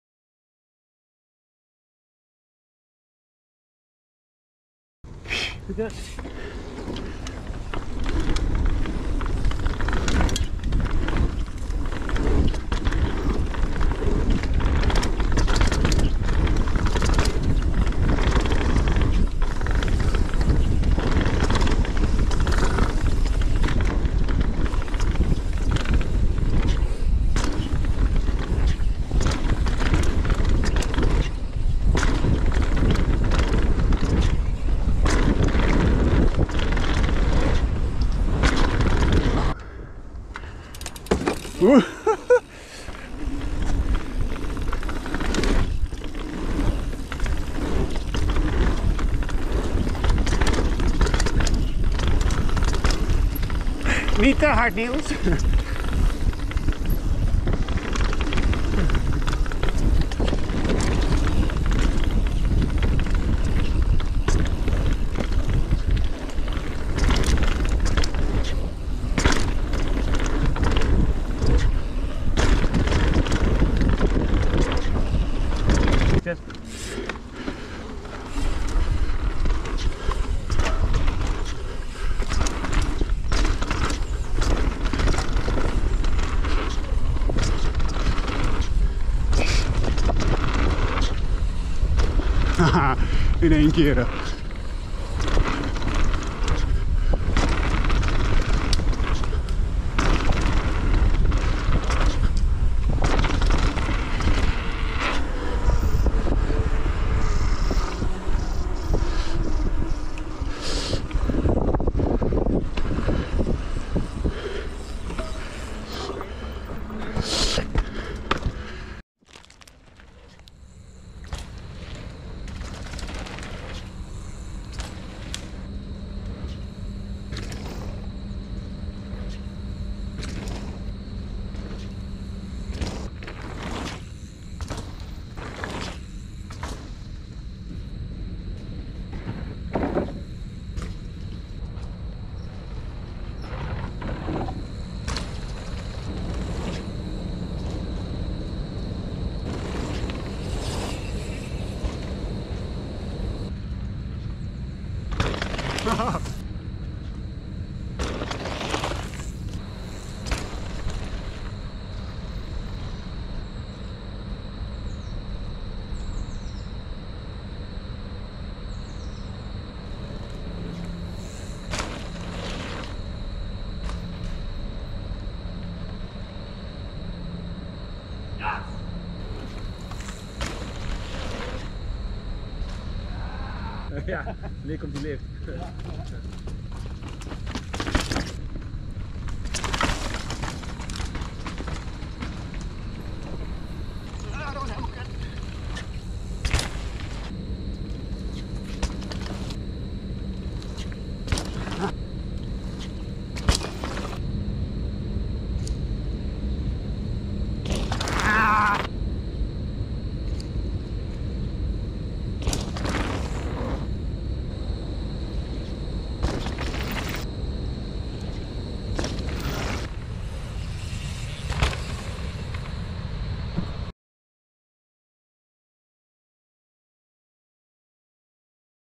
Bikepark Spaarnwoude Propain Tyee Eerste keer is altijd weer wennen
Met Mark en Niels, heerlijk weer, grond niet te nat, perfect voor weer inkomen